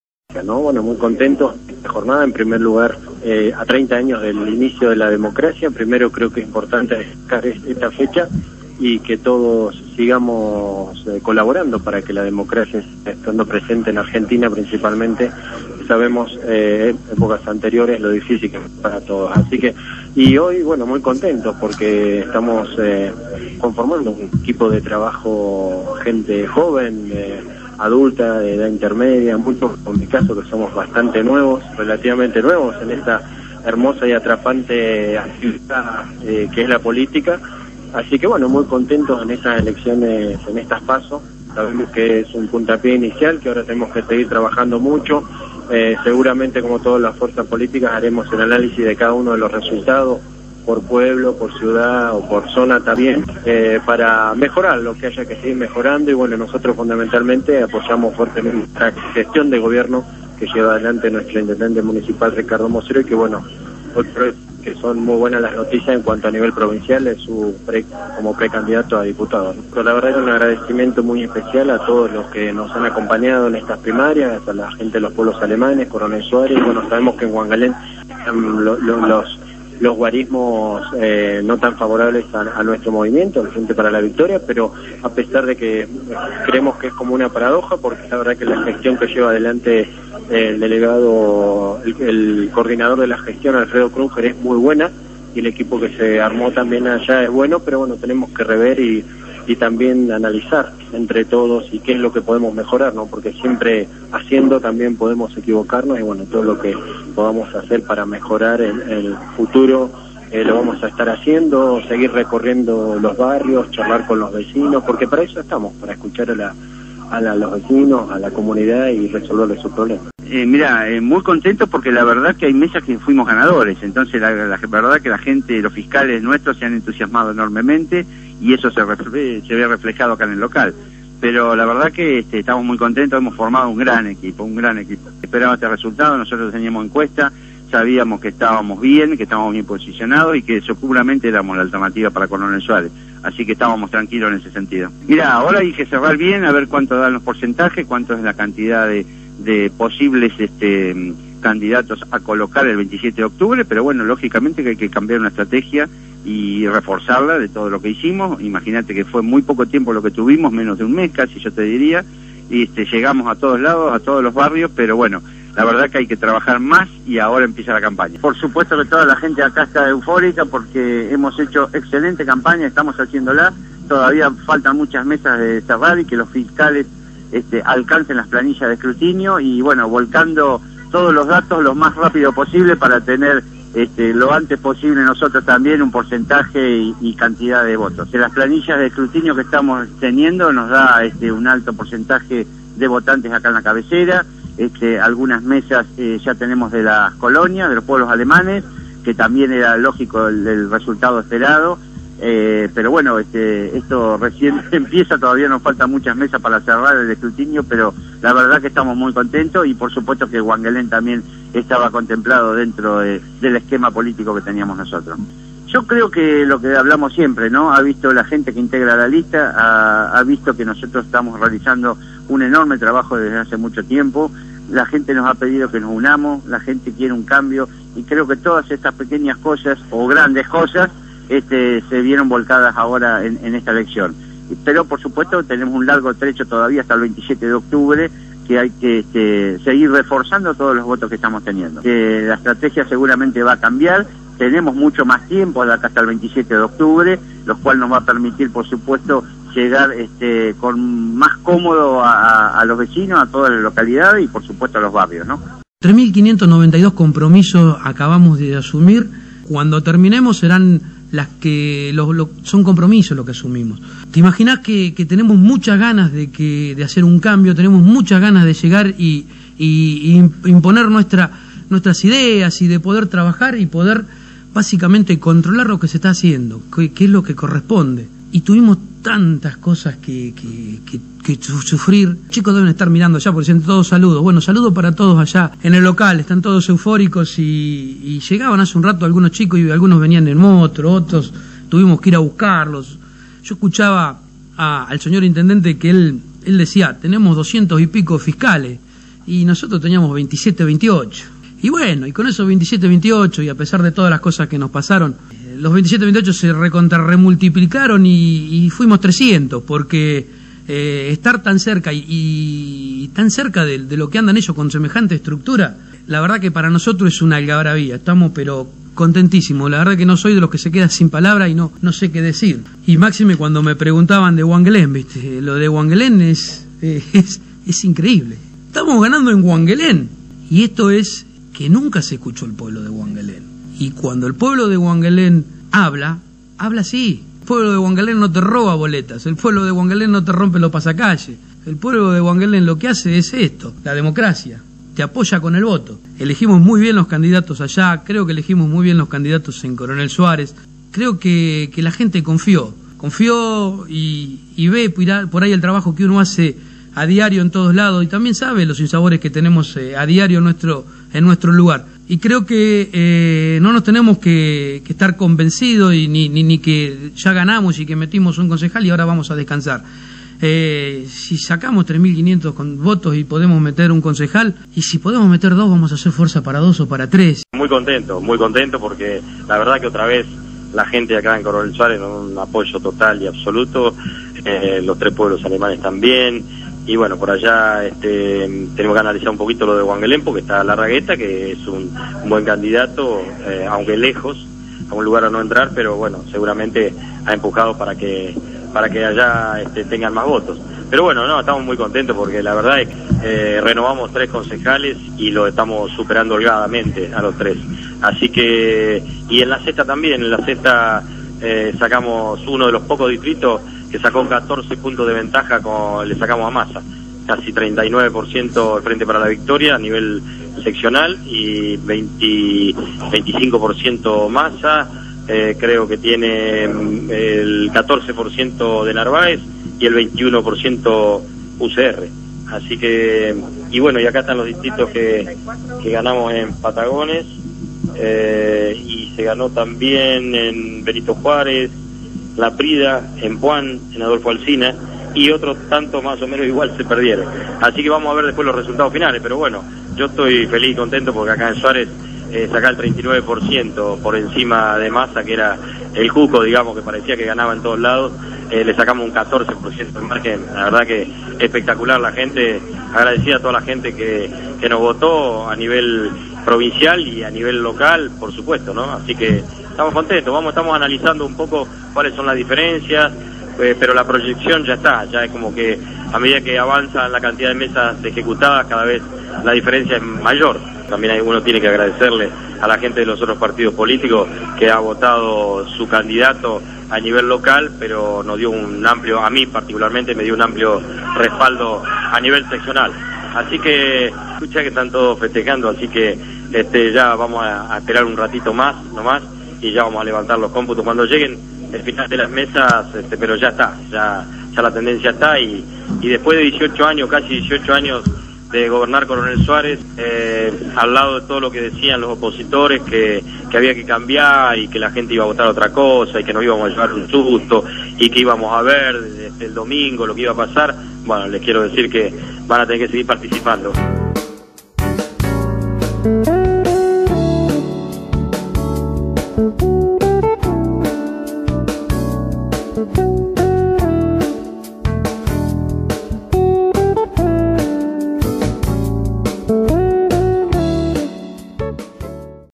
"Ajustado Triunfo del Frente para la Victoria en Coronel Suárez"
101 mesas escrutadas en el Distrito de Coronel Suárez:

1º Frente para la Victoria: Claudio Holzmann - 6940 votos.
2º Unos: Guillermo Recalde - 5110 votos.
3º Frente Cívico y Social UCR: Carlos Delgado - 4287 votos.
4º Frente Renovador: Fabián González - 3725 votos.
5º Unidos por la Libertad y el Trabajo: Ricardo Álvarez - 1841 votos.
6º Frente de Izquierda: Rubén Allende - 920 votos.
7º Frente Cívico y Social: Pablo Diaz - 645 votos.
8º Unión con Fe: Carlos Cannes - 470 votos.

Votos en Blanco: 1.200
Votos nulos: 497

Huanguelén: 14 mesas escrutadas.

Frente Renovador: Fabian Gonzalez - 1028 votos.
Frente para la Victoria: Claudio Holzmann - 814 votos.
Frente Civico y Social, UCR: Carlos Delgado - 652 votos.
UNOS:Guillermo Recalde - 397 votos.
Ricardo Alvarez: 175 votos